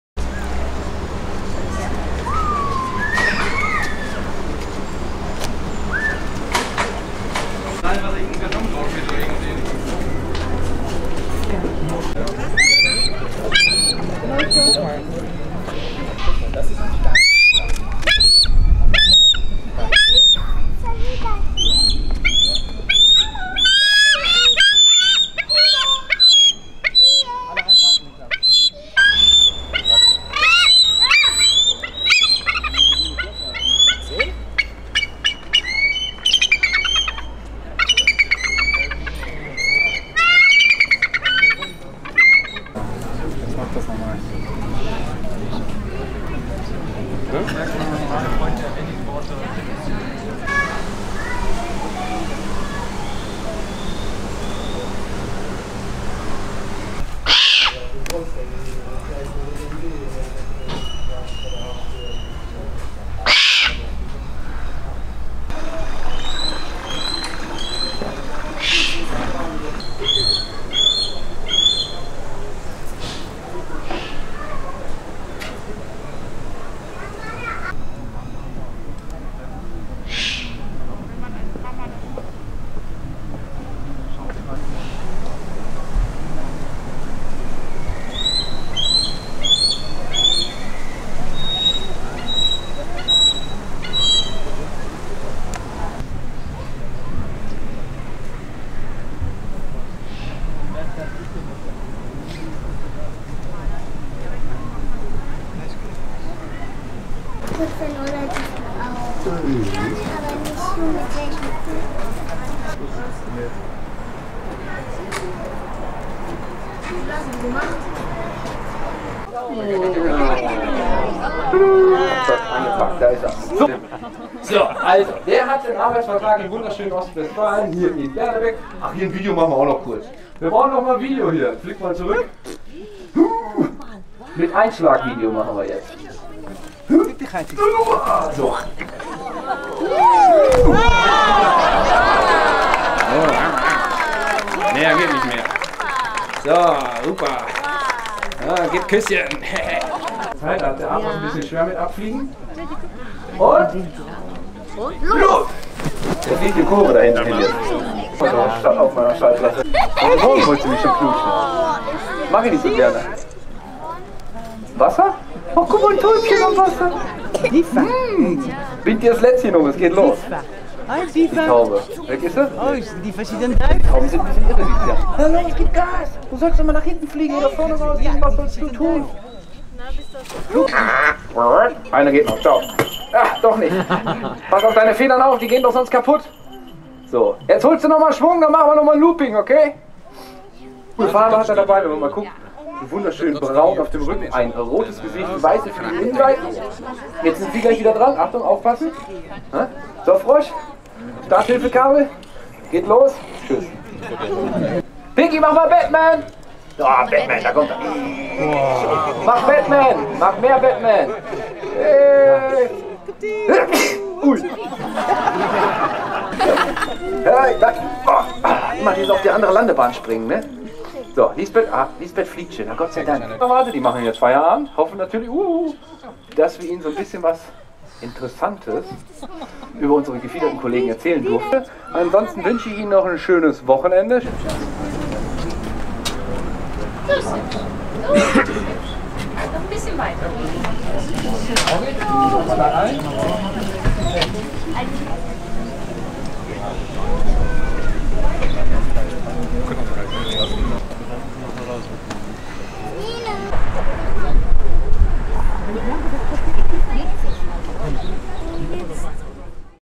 Adlerwarte Berlebeck bei Detmold
Besuch in der Adlerwarte Berlebeck bei Detmold. Die Flugschau der Greifvögel startet täglich um 11, 15 und 16:30 Uhr!  Die sollte man nicht verpassen. Weißkopfseeadler, Schreiseeadler, Gänsegeier, Kappengeier, Rotmilan, Gaukler und Falken, alles Akrobaten der Lüfte, sie sind in der Flugschau zu sehen.